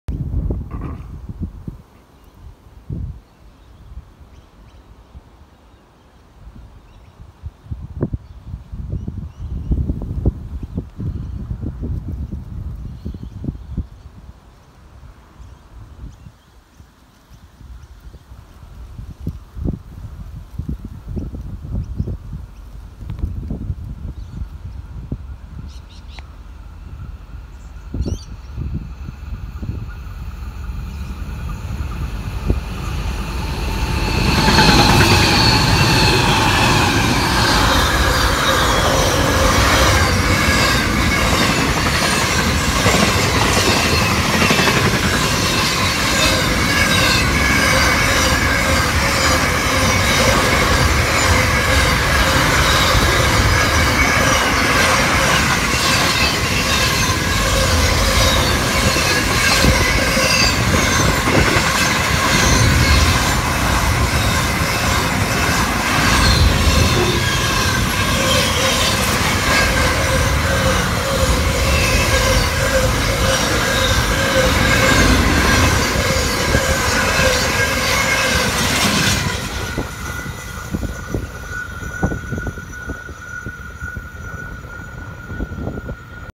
7BM4 hauled by NR35/121/53/113 at minto
Location: minto (opp 1 steel site)
Time: 15.06 (day light saving time)
Date: 7.10.18
Speed: 82.3km/h